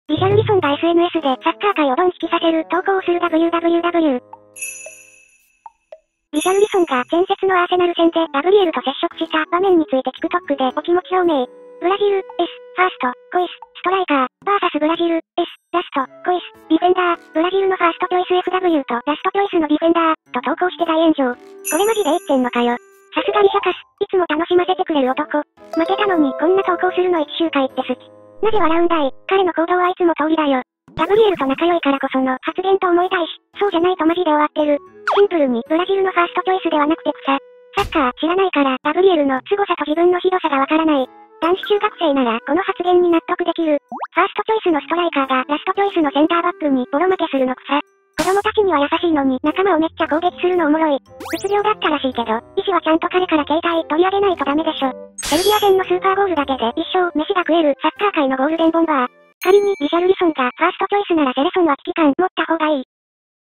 リシャルリソンがSNSでサッカー界をドン引きさせる投稿をするwww
おもろすぎ